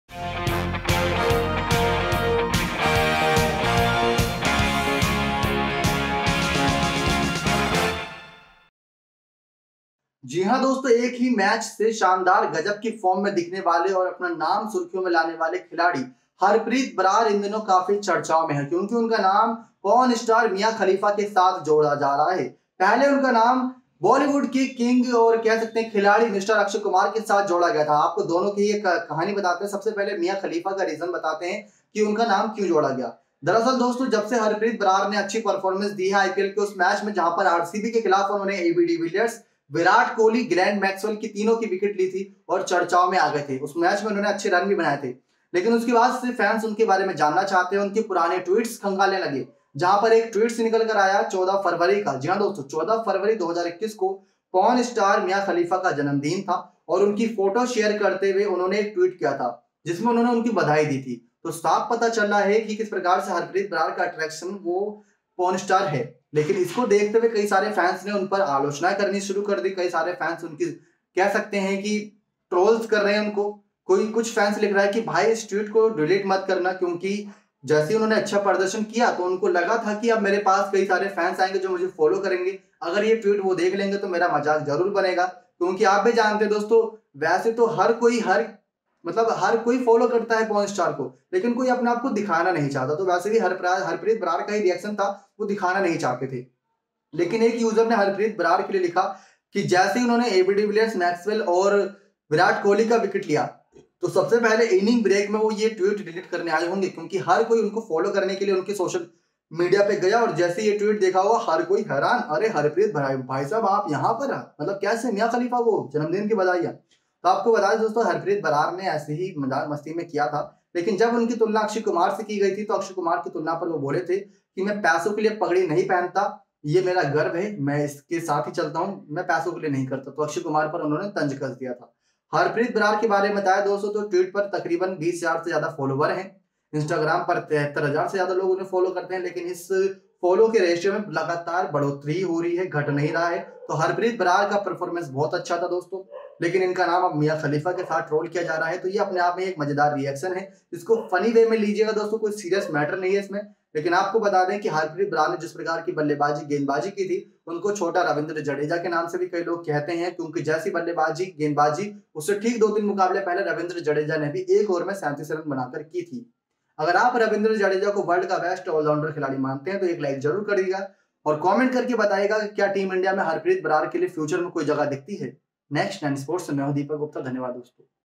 MIA KHALIFA और PUNJAB KINGS के खिलाड़ी HARPREET BRAR के बीच कनेक्शन ? वायरल हुआ पुराना TWEET
HARPEET ने 14 फरवरी को मिया खलीफा को उनके जन्मदिन के शुभअवसर पर उन्हें शुभकामनाएं दी थी, लेकिन अब इसी ट्वीट को लेकर लोग उनको ट्रोल कर रहें है...